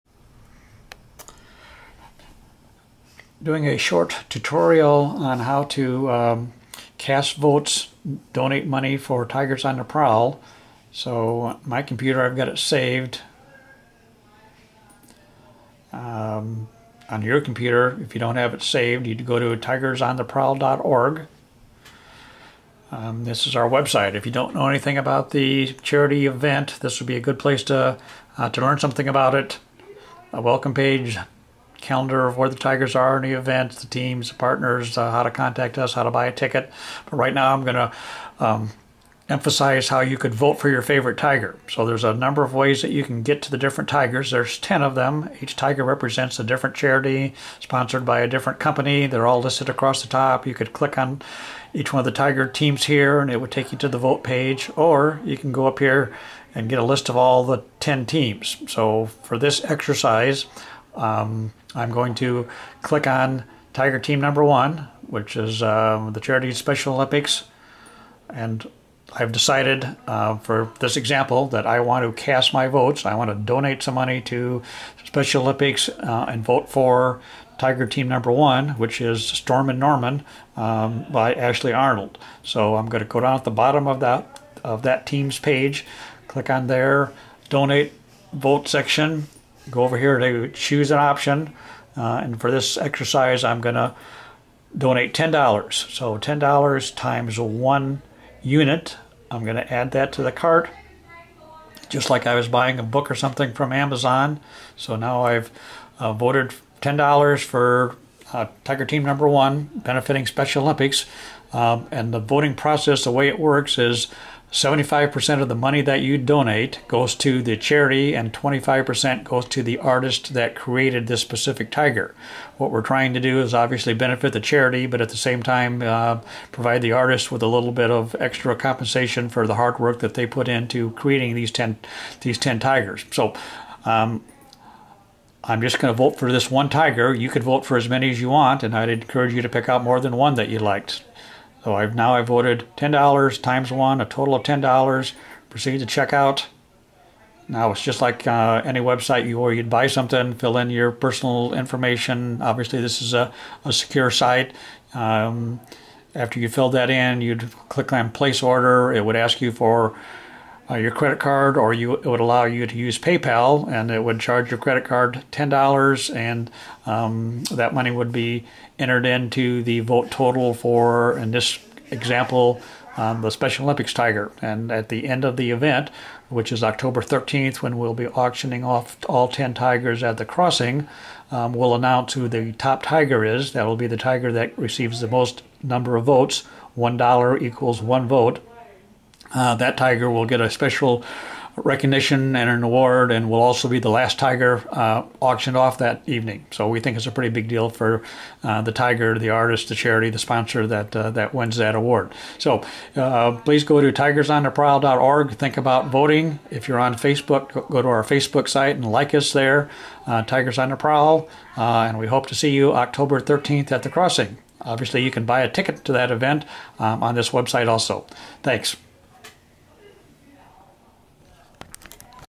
Tigers on the Prowl Tutorial on Voting
Want to vote for your favorite tiger? Learn how here.